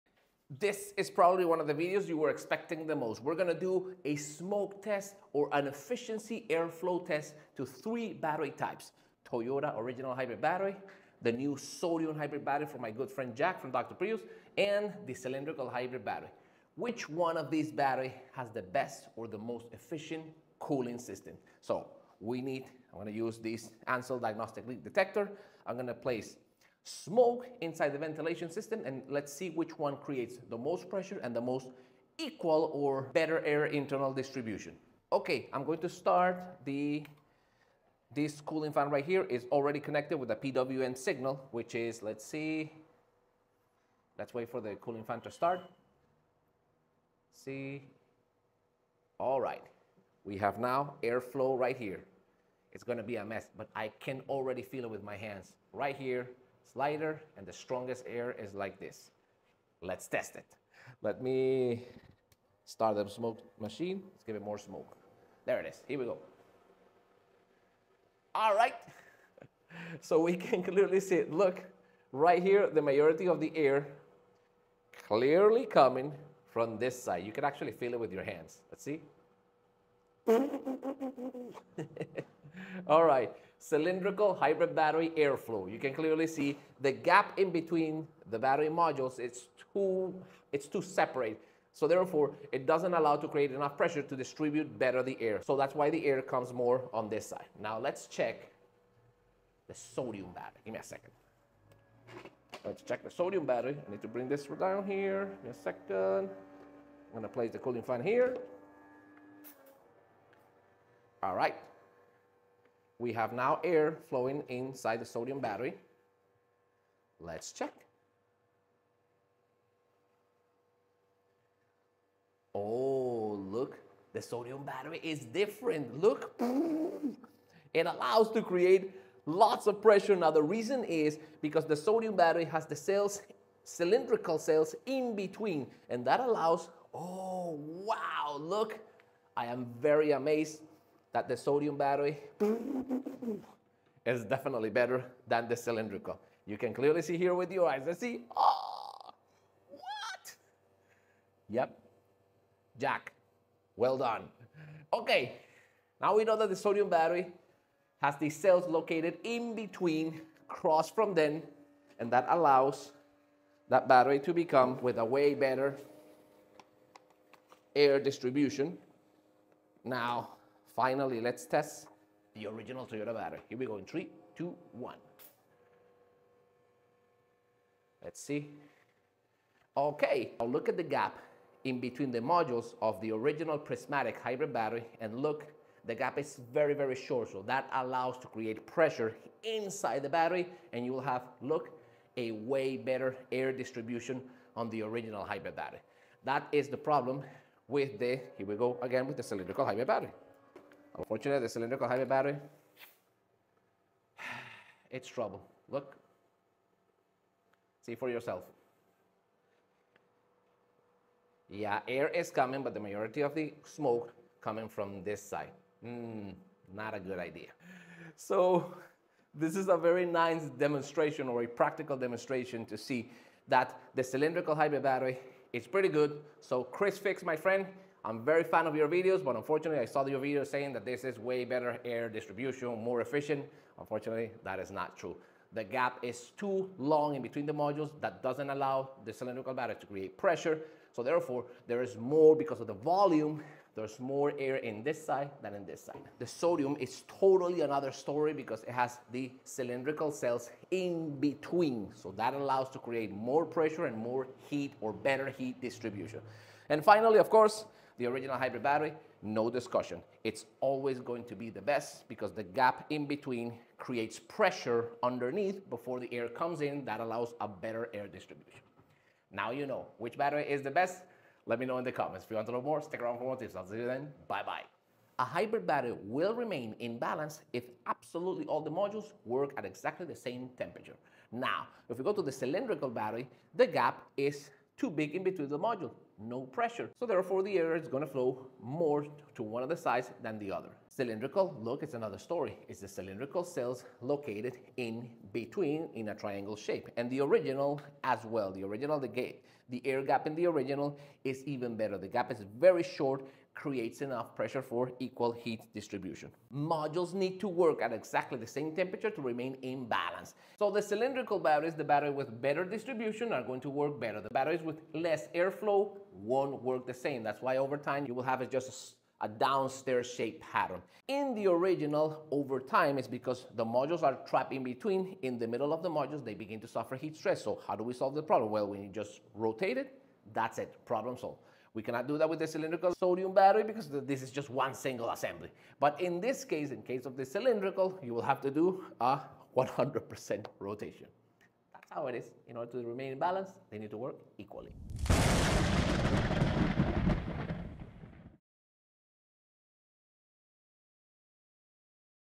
Hybrid Battery Airflow Efficiency Test Toyota vs Sodium vs Cylindrical
Hybrid Battery Airflow Efficiency Test: Toyota OEM vs Aftermarket Options

In this video, we demonstrate the efficiency of airflow in three different hybrid battery designs for Toyota vehicles:

1️⃣ Original Toyota Hybrid Battery: Known for its superior and balanced heat exchange system, it creates built-in air pressure, ensuring even airflow distribution for long-term performance.

2️⃣ Aftermarket Sodium Hybrid Battery: Featuring cylindrical cells in a cross pattern, this design also builds up air pressure and achieves a good heat exchange, making it a reliable option.

3️⃣ Aftermarket Cylindrical Hybrid Battery: Unfortunately, this design has significant air gaps, which fail to build proper air pressure. The uneven airflow accelerates battery degradation over time, leading to imbalance and reduced efficiency.

Watch this hybrid battery airflow masterclass to see how each design performs and learn why proper airflow matters for hybrid battery longevity and efficiency.